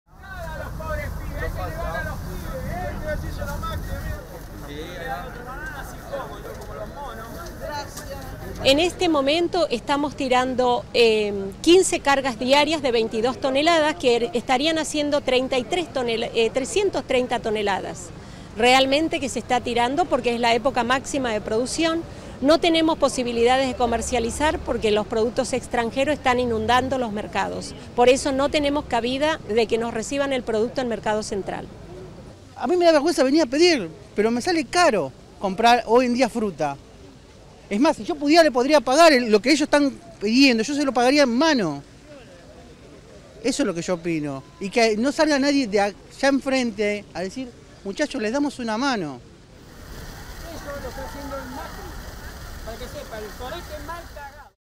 Argentine farmers protest by giving away 30 tons of bananas
Outraged by the high levels of banana imports, Argentine agricultural producers gave away more than 30.000 kilos of the fruit on Wednesday, an act that made for long queues on Buenos Aires’ emblematic square, Plaza de Mayo.